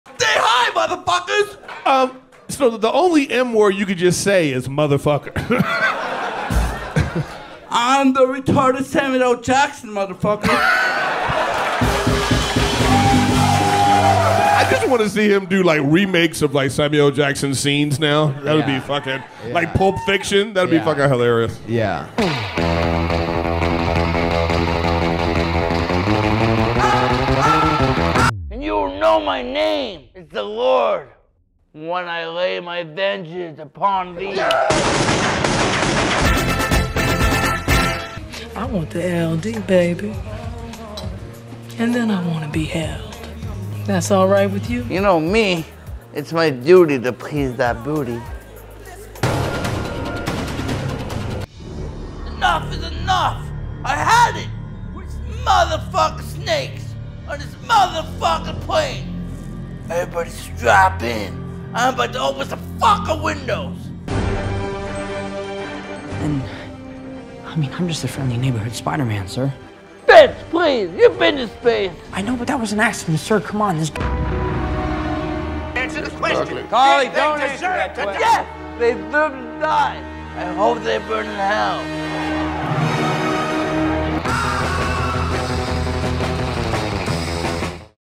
Jared Nathan is Samuel L. Jackson
When Jared Nathan visited our podcast to talk about Kill Tony, we couldn't resist making Erik Griffin's vision come true by recreating these iconic Samuel L. Jackson scenes.